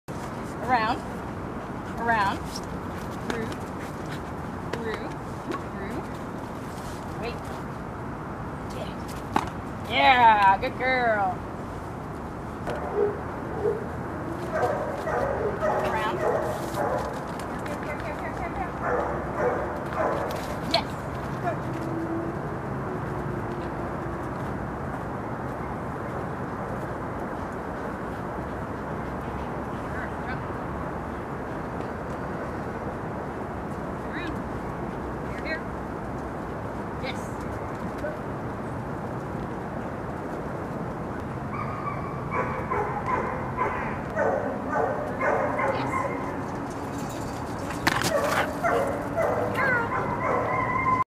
lilly available for adoption Whatcom Humane Society *Adopted*
located at Whatcom Humane Society Bellingham, WA
stray, approx 4yo female.  Pitbull/visla?  3rd time playing disc.  Very friendly, good with other dogs, kids over 8.